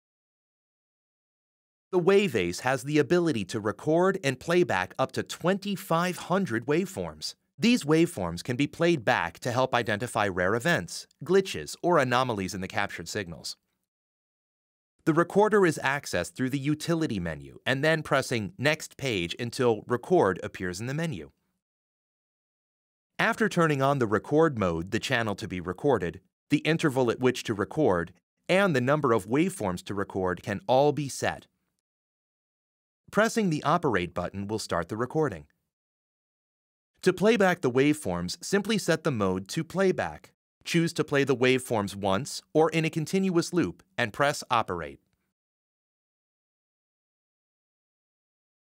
Using the Waveform Sequence Recorder
The WaveAce has the ability to record and playback up to 2,500 waveforms.